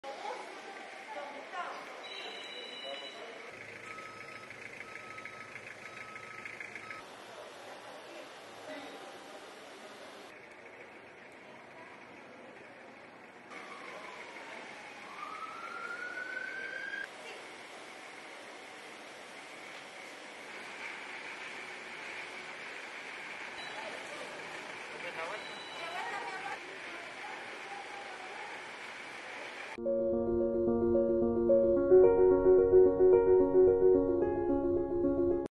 Hospital El Salvador brinda alta médica a 18 personas que superaron al COVID-19
AltasCOVID19 || Para las personas que han logrado superar la enfermedad y para el personal de primera línea, el Hospital no solo representa un centro de atención médica especializada, sino esperanza para poder reunirse de nuevo con sus familias.